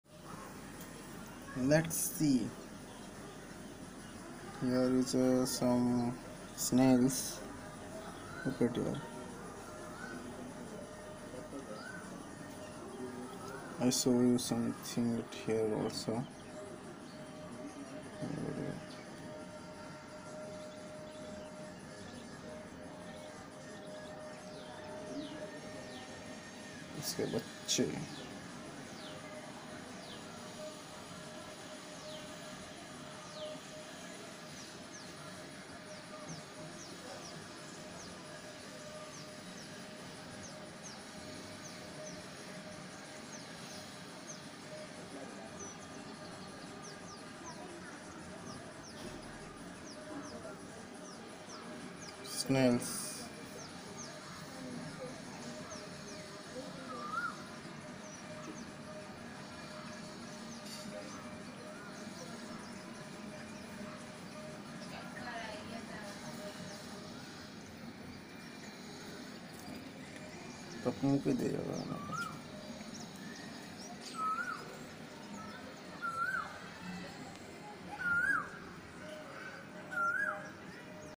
Baby Snail 🐌 in our port
Snails born in the port
They are eat mango seed